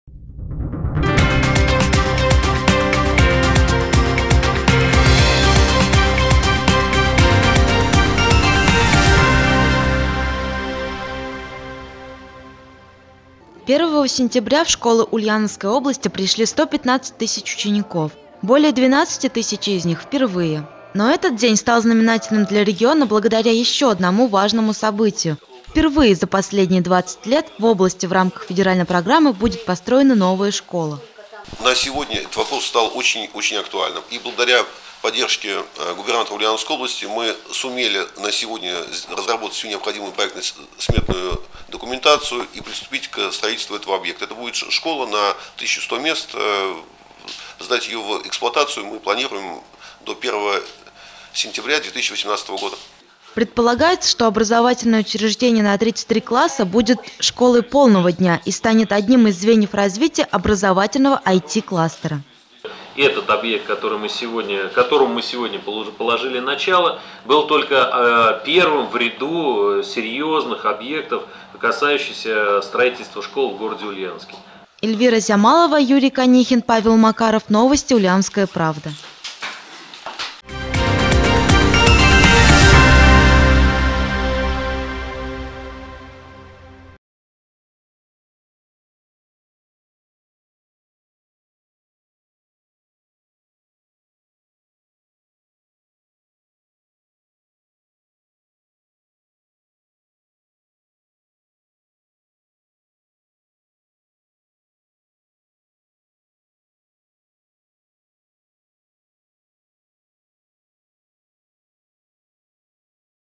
Первого сентября 2018 года в регионе откроется первая за двадцать лет новая школа
Школа на 1100 мест будет построена в Ульяновске, в микрорайоне «Искра». Предполагается, что образовательное учреждение будет школой полного дня и станет одним из звеньев развития образовательного IT-кластера, создаваемого на базе УлГУ и УлГПУ. Обучение в новой школе на 33 класса будет организовано в одну смену.
Сметная стоимость объекта (по расчетам IV квартала прошлого года) -797377,69 тысячи рублей. По словам главы администрации города Алексея Гаева, сдача объекта в эксплуатацию планируется до 1 сентября 2018 года.